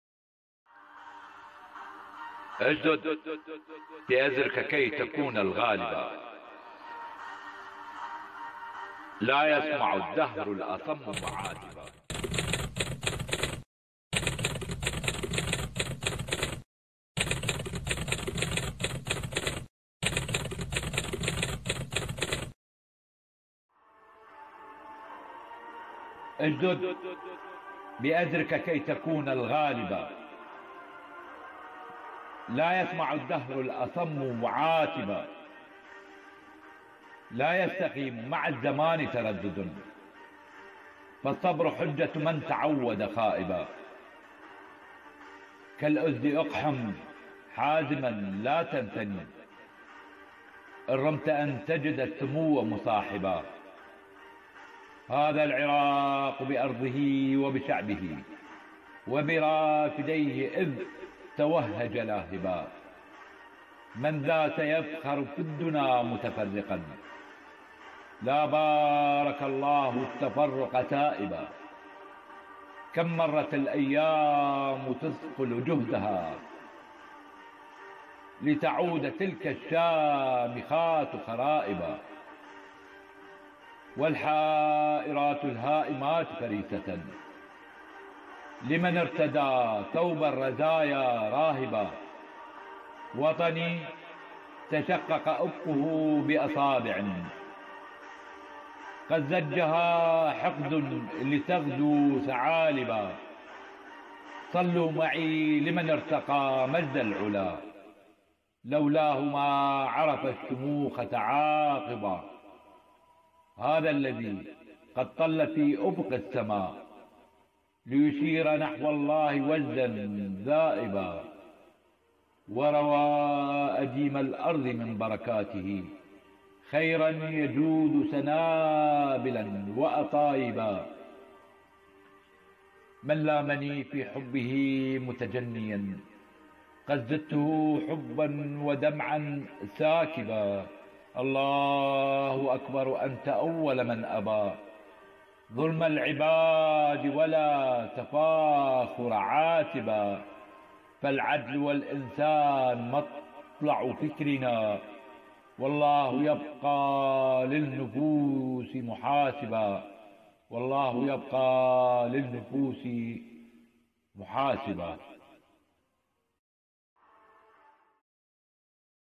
قصيدة (أشددْ بأزركَ ) للشاعر والباحث كريم مرزه الاسدي . القاء مالك عبدالقادر المهداوي.
قصيدة- اشددْ بأزركَ - الشاعر والباحث كريم مرزة الاسدي .
إلقاء ... مالك عبدالقادر المهداوي,
كتب الشاعر والباحث كريم مرزة الاسدي, قصيدة - أشدد بأزرك , تضامناً مع ثوار تشرين وحركات نازل أخذ حقي فقد اثبت احرار العراق حبهم وحقق شباب العراق غاياتهم في التغيير.  نازل_أخذ_حقي,,  رحمة رياض , بغداد , ساحة التحريرثورة تشرين,
النصر لشعبنا العراقي العظيم، وأمتنا المجيدة؛الأمم الخالدة لا تعرف اليأس والقنوط، والدنيا دول- قصيدتي عن العراق والأمة الكامل :
أشددْ بأزركَ كي تكونَ الغالبا *** لا يسمعُ الدّهرُ الأصمّ ُمعاتبا
لا يستقيمُ مع الزّمان ِتردّدٌ فالصبرُ حجَــة ُمنْ تعوّدَ خائبا
كالأ ُسْدِ اقحمْ حازماً،لاتنثني***إنْ رمتَ أنْ تجدَ السّموَّ مُصاحبا
هـــــذا العراقُ بأرضِهِ وبشعبهِ *** وبرافديهِ؛ إذْ توهّـــجَ لاهبا
مَنْ ذا سيفخرُ في الدنى متفرقاً *** لا بـــاركَ اللهُ التـّقرقَ سائبا
كمْ مـــــــرّتِ الأيّامُ تثقلُ جُهدَها*** لــتعودَ تلكَ الشامخاتُ خرائبا
والحــائراتُ الهائماتُ فريسةً ً ***لمنْ ارتدى ثوبَ الرزايا راهبا
وطني تشقـّقَ أفقـُـهُ بأصابع ٍ *** قد زجـّها حقــــــدٌ لتغدو ثعالبا
صلـّوا معي لمنْ ارتقى مجدّ العلا ** لولاهُ ما عرفَ الشموخُ تعاقبا
هذا الذي قدْ طلَّ منْ أفقِ السمـــا *** ليشيرَ نحوّ اللهِ وجداً ذائبـا
وروى أديمَ الأرضِ منْ بركاتـــهِ *** خيراً يجودُ سنابلاً وأطايبا
منْ لامني في حبّــهِ متجنـياً *** قــدْ زدتـُهُ حبّــاً ودمعـاً ساكبا
اللهُ أكبرُ أنـــــتَ أولُ منْ أبــى *** ظلمَ العبـــــادِ ولا تفاخرُ عاتِبا
فالعدلُ والإنسانُ مطلــعُ فكرنا واللهُ يبقى للنفــوس ِمُحـاسبا